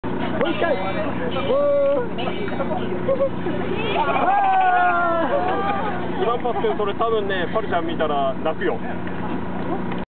鯱、パルちゃんを斬る
2010年7月24日名古屋VS清水＠瑞穂の試合後、辻斬りをする剣豪。